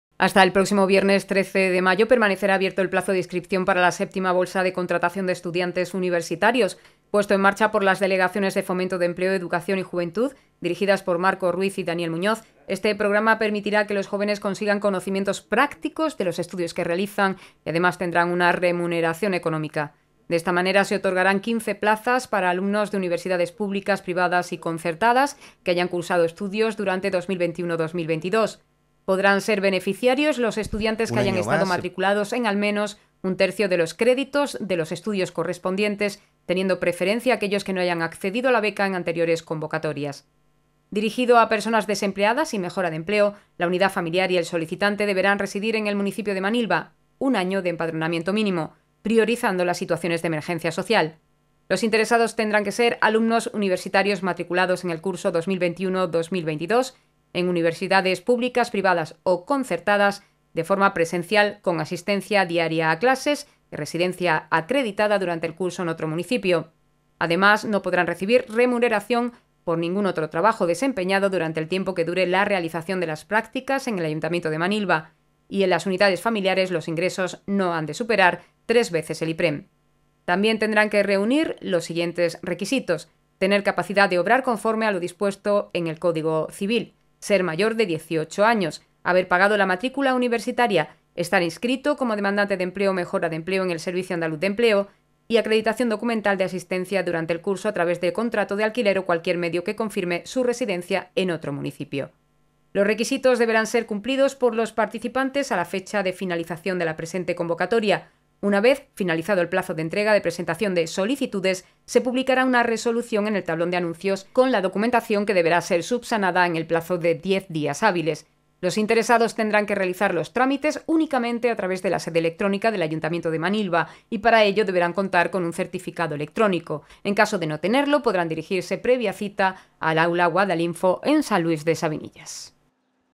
Bolsa de empleo para universitarios
Hasta el próximo viernes 13 de mayo permanecerá abierto el plazo de inscripción para la séptima bolsa de contratación de estudiantes universitarios.

Puesto en marcha por las Delegaciones de Fomento de Empleo, Educación y Juventud, dirigidas por Marcos Ruiz y Daniel Muñoz, este programa permitirá que los jóvenes consigan conocimientos prácticos de los estudios que realizan y, además, tendrán una remuneración económica.

De esta manera, se otorgarán 15 plazas para alumnos de Universidades públicas, privadas y concertadas, que hayan cursado estudios durante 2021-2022.

Podrán ser beneficiarios los estudiantes que hayan estado matriculados en, al menos, un tercio de los créditos de los estudios correspondientes, teniendo preferencia aquellos que no hayan accedido a la beca en anteriores convocatorias.

Dirigido a personas desempleadas o en mejora de empleo, la unidad familiar y el solicitante deberá residir en el municipio de Manilva (un año de empadronamiento mínimo), priorizando las situaciones de emergencia social.

Los interesados tendrán que ser alumnos universitarios matriculados en el curso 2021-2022 en Universidades públicas, privadas o concertadas, de forma presencial (con asistencia diaria a clases y residencia acreditada durante el curso en otro municipio). Además, no podrán recibir remuneración por ningún otro trabajo desempeñado durante el tiempo que dure la realización de las prácticas en el Ayuntamiento de Manilva; y, en las unidades familiares los ingresos no han de superar 3 veces el IPREM.

También tendrán que reunir los siguientes requisitos:
- Tener capacidad de obrar conforme a lo dispuesto en el Código Civil.
-Ser mayor de dieciocho años.
-Haber pagado la matricula universitaria.
-Estar inscrito como demandante de empleo o mejora de empleo en el Servicio Andaluz de Empleo.
- Acreditación documental de asistencia durante el curso, a través de contrato de alquiler o cualquier medio que confirme su residencia en otro municipio.

Los requisitos deberán ser cumplidos por los participantes a la fecha de finalización de la presente convocatoria. Una vez finalizado el plazo de entrega de presentación de solicitudes, se publicará una Resolución en el tablón de anuncios con la documentación que deberá ser subsanada en el plazo de diez días hábiles. Los interesados tendrán que realizar los trámites, únicamente, a través de la sede electrónica del Ayuntamiento de Manilva y para ello, deberán contar con un certificado electrónico. En caso de no tenerlo, podrán dirigirse, previa cita, al Aula Guadalinfo, en San Luis de Sabinillas.